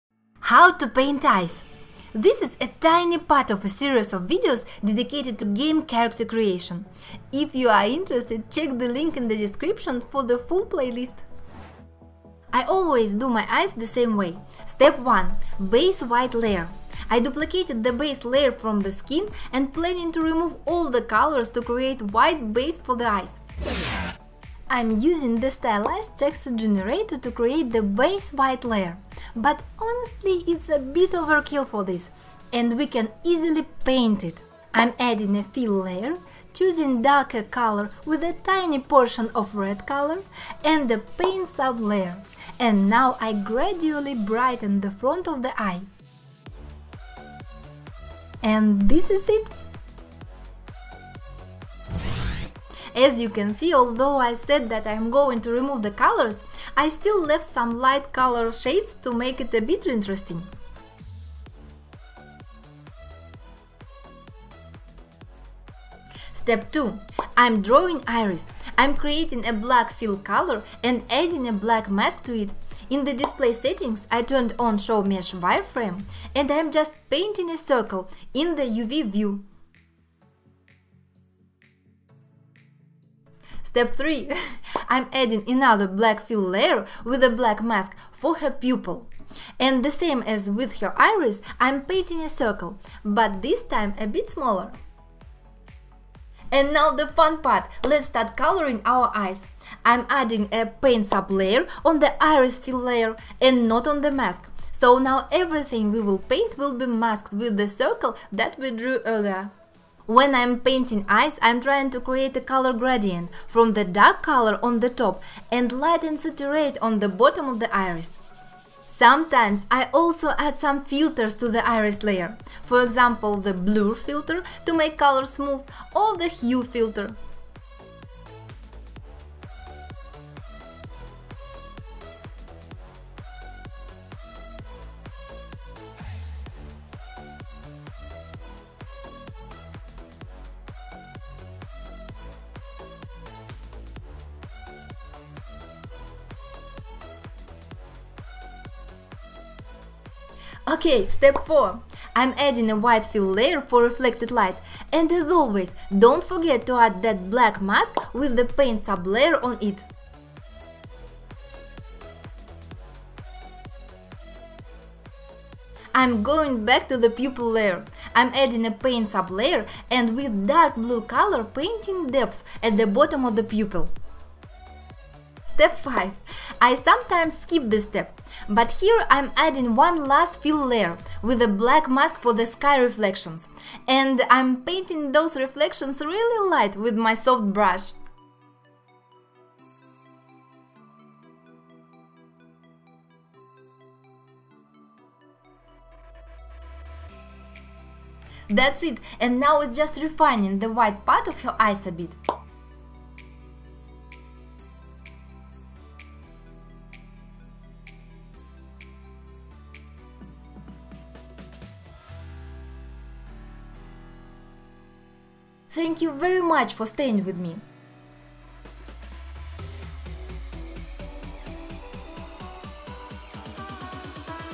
How To Draw (Paint) Eyes? In Substance Painter.
✨Hello!✨This is a tiny part of a series of videos dedicated to game character creation. But I thought this part turned out to be quite interesting and deserves a separate short video.

We are posting the latest news there. 😊

⌚ Timestamps:
00:00 How to draw eyes?
00:14 Step 1
01:05 Step 2
01:23 Step 3
02:30 Step 4
02:55 Step 5
03:45 Ending